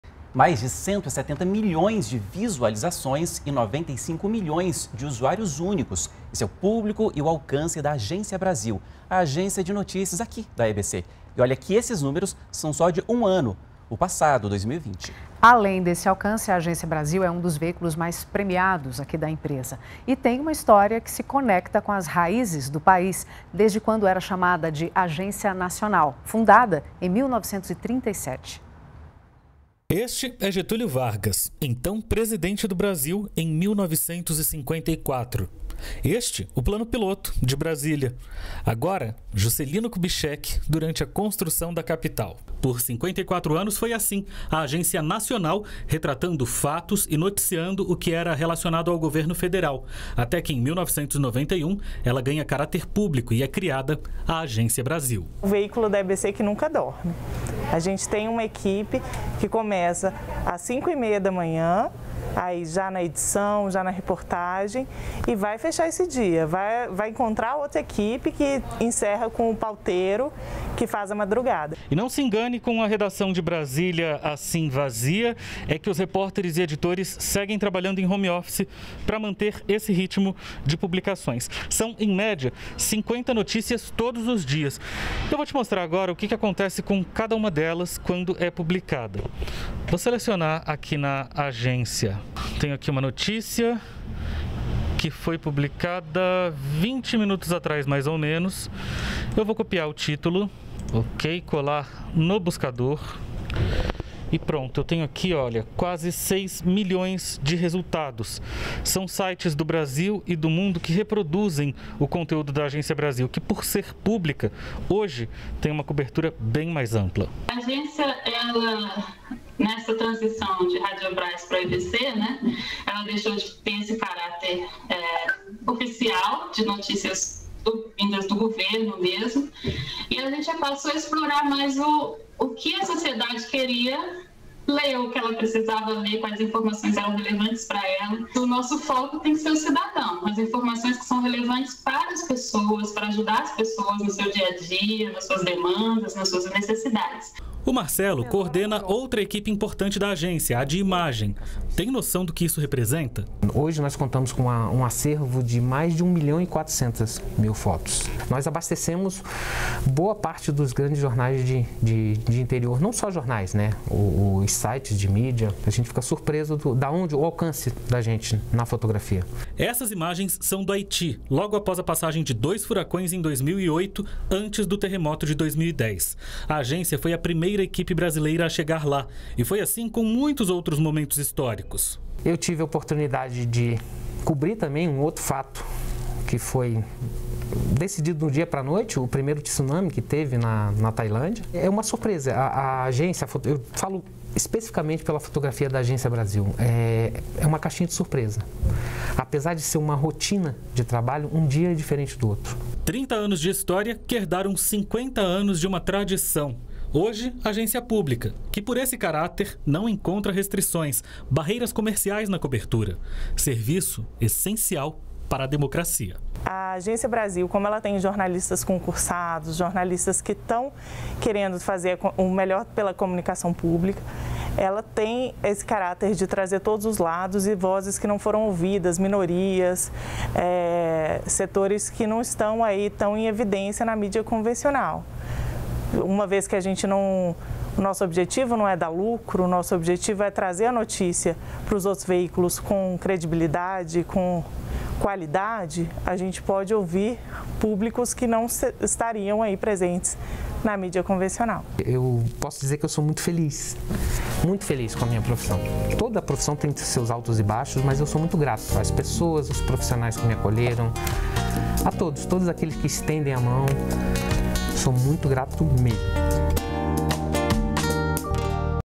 Agência Brasil: jornalismo público para o Brasil e o mundo
Mais de 170 milhões de visualizações e 95 milhões de usuários únicos. Esse é o público e o alcance da Agência Brasil, a agência de notícias da EBC. E, olha que esses números são só de um ano, 2020. Além desse alcance, a Agência Brasil é um dos veículos mais premiados da empresa e tem uma história que se conecta com as raízes do país, desde quando era chamada de Agência Nacional, fundada em 1937.